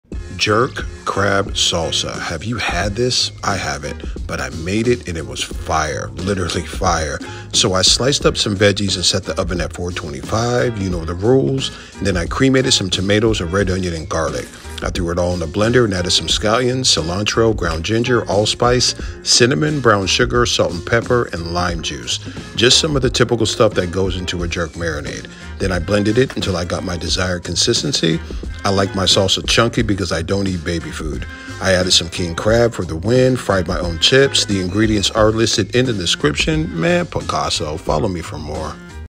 Jerk Crab Salsa 🏆
Ingredients:

4-6 oz King Crab (lightly broken up) – Optional
10 Roma Tomatoes (halved)
1-2 Scotch Bonnet Peppers (or habaneros)
2 tbsp Extra Virgin Olive Oil
1 tsp Salt
1 tsp Pepper
1 tsp Dark Brown Sugar
1 Garlic Bulb
Lime Juice (1 lime)
1 Small-Medium Red Onion
2 Scallions (chopped)
¼ cup Fresh Cilantro (chopped) – Save a teaspoon for garnish
1/4 tsp Ground Cinnamon
1/4 tsp Ground Allspice
1/2 tsp Ground Ginger

Instructions:
In a medium-sized skillet over medium-high heat, add a dash of olive oil, crab, and a pinch of salt and pepper to taste. Cook the crab for about 1 minute, then remove it from the heat and set it aside.

Using a cutting board, carefully cut the tops off the tomatoes, then halve them.

Next, cut the red onion into quarters for quicker and more even roasting.

In a large mixing bowl, rub the tomato halves, red onion quarters, and scotch bonnet peppers with a little olive oil.

Take one whole garlic bulb, cut off the top to expose the cloves inside. Season it with salt and pepper, and drizzle a bit of olive oil over it. Wrap the garlic bulb in aluminum foil, preparing it to be placed on the same baking sheet with the other vegetables for roasting in the next step.

Place the oiled tomatoes, red onions, and scotch bonnet peppers on a baking sheet lined with parchment paper. Roast for approximately 20-30 minutes (ovens may vary) until they are charred and blistered, turning them halfway through. Leave the garlic bulb in the foil as it will naturally roast on its own.

Once done, let the vegetables cool for a few minutes. Please use caution when opening the foil with the garlic, as it may still be extremely hot.

In a food processor, pulse the tomatoes, onions, peppers, garlic, scallion, most of the cilantro, allspice, cinnamon, ground ginger, lime juice, dark brown sugar, and salt & pepper until they reach your desired consistency.

Pour the salsa into a large serving bowl, add the crab, and gently fold to incorporate all the ingredients into the crab.

Taste and adjust.